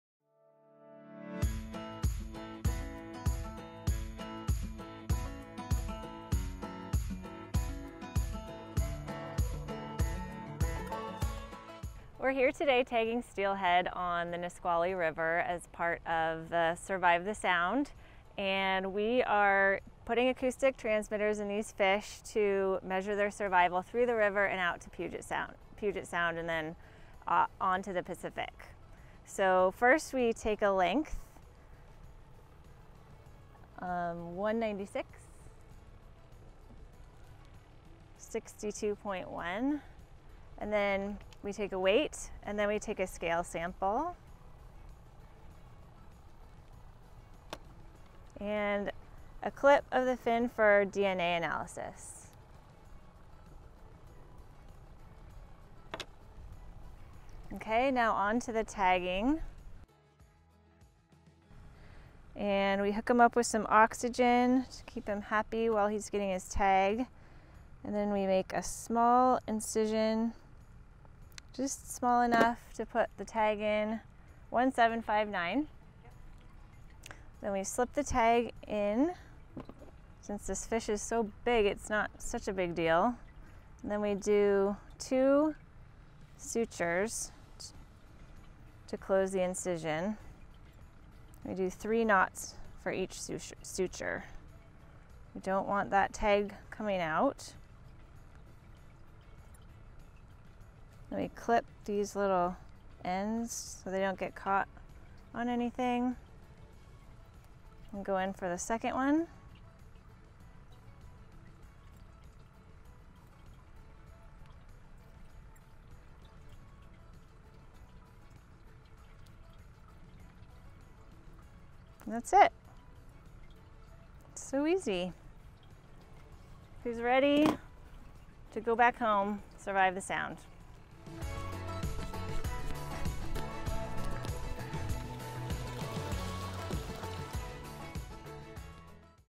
Tagging Steelhead Smolts on the Nisqually River
Megan Moore, Research Fisheries Biologist from the Northwest Fisheries Science Center, teaches how she tags out-migrating steelhead smolts. These fish are tagged and tracked to better understand juvenile steelhead survival in Puget Sound.

Track these fish along their migration at SurvivetheSound.org